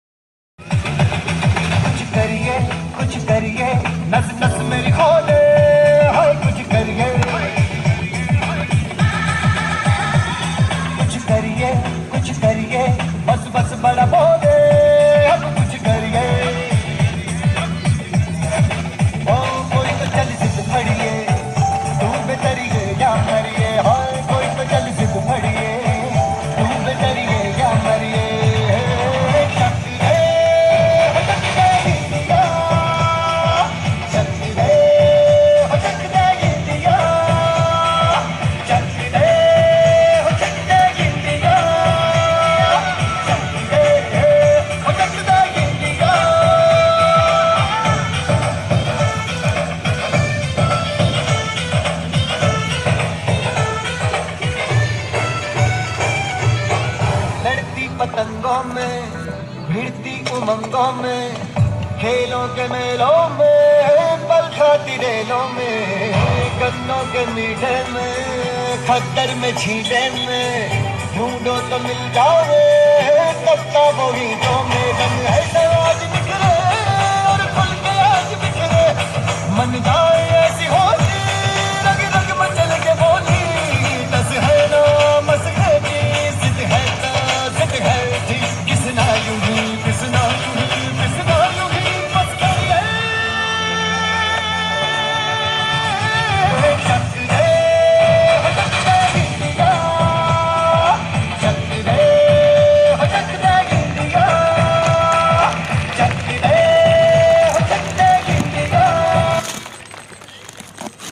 HAL VIDYALAYA KANPUR
Nice dance by school students